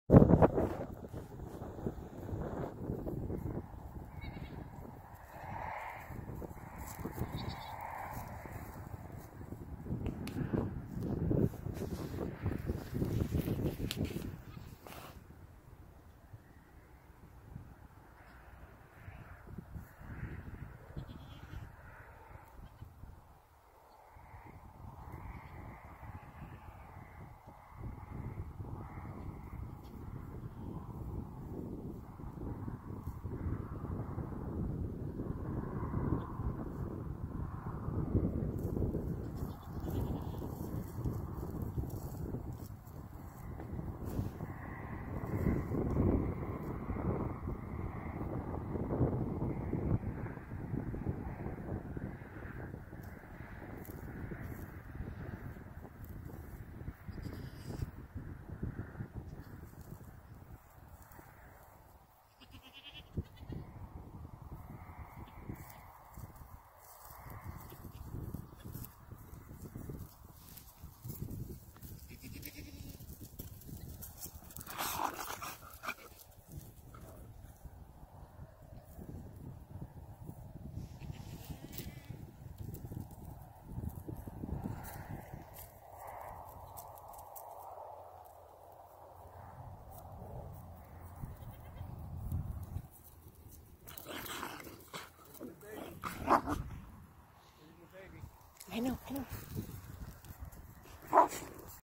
Llamas Guarding Kiko Goat Giving Birth in Open Pasture
Curious about how llamas work with the herd?
Watch them with a doe kidding on open pasture...
Our rental pastures are challenging for the guardians during kidding because they are wide open. There are no shrubs or trees for a doe vulnerable at delivery to seek cover. We have ground and aerial predators. The llamas recognize this and stay very close to any animal that seems vulnerable - or that separates from the herd. In this video Agatha, one of our New Zealand does, is kidding twins, and she reminds them that close is nice, but not TOO close. Kiko kids are very vigorous at birth, and with multiples, the firstborn can wander a bit while their siblings are being born. When that happens, the llamas stay with them until their dam finishes and can collect them all. Notice also the way the guardians - llamas and livestock guardian dogs - work together. Llamas generally stay very close to the herd, while the dogs disperse and guard the perimeter. Of course, birth means clean up, so the dogs come in. Bud, the llama, makes it clear to Willie that he is not allowed near until Bud says so. Will E talks back, but respects the boundaries. Both llamas in this video are geldings.